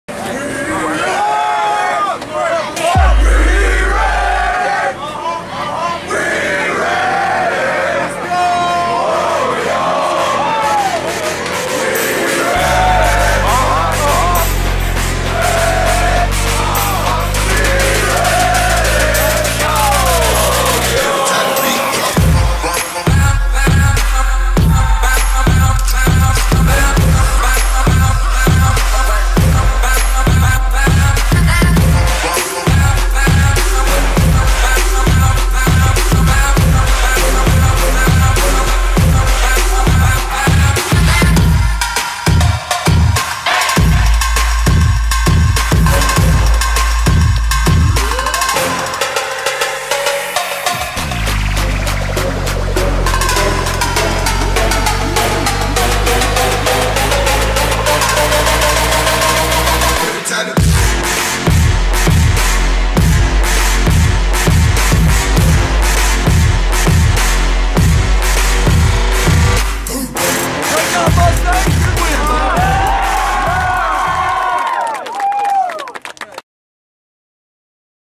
CAC Football Week 1 2019
CAC beat Mayflower 48-0 on August 30, 2019.